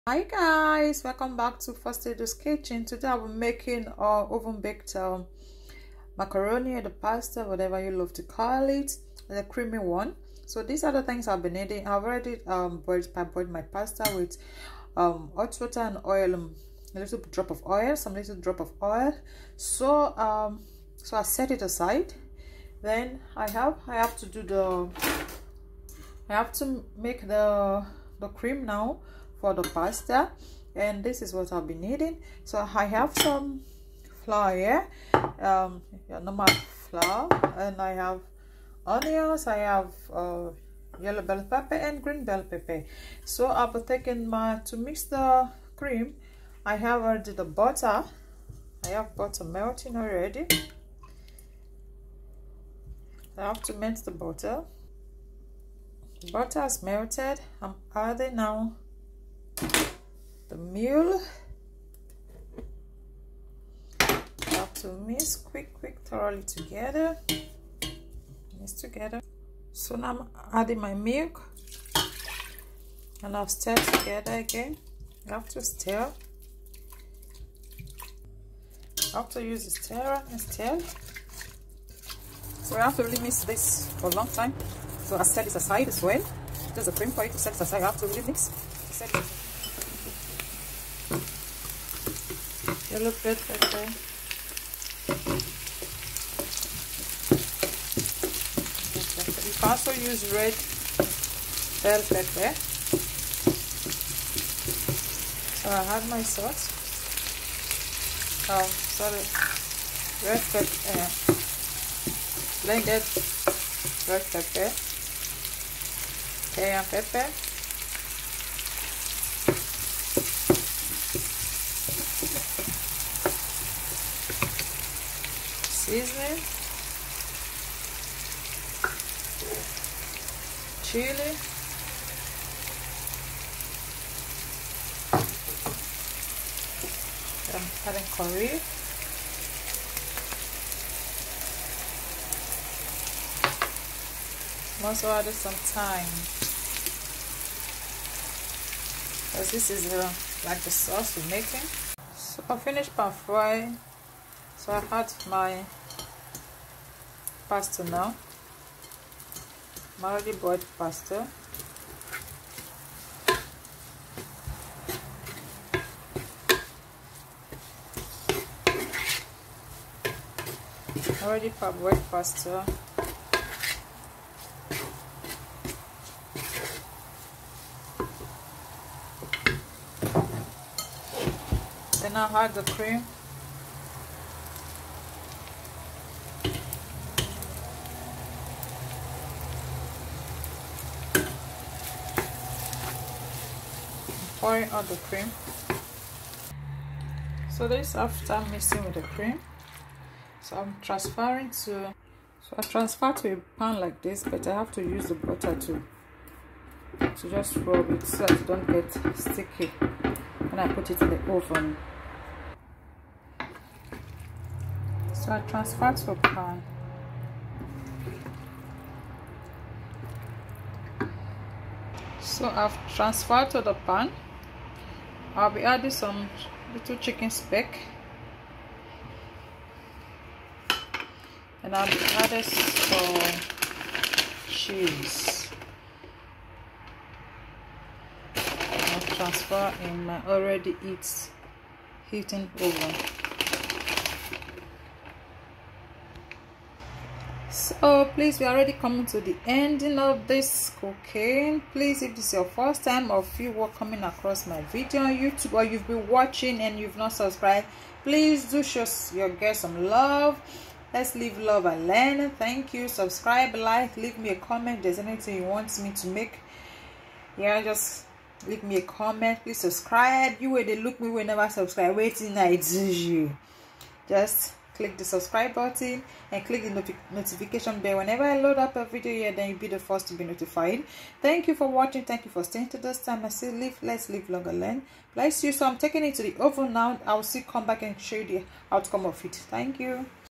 creamy oven pasta 😋
it's made of pasta,meal, oil,, green bell pepper  and yellow Bell pepper, onions ,salt and seasoning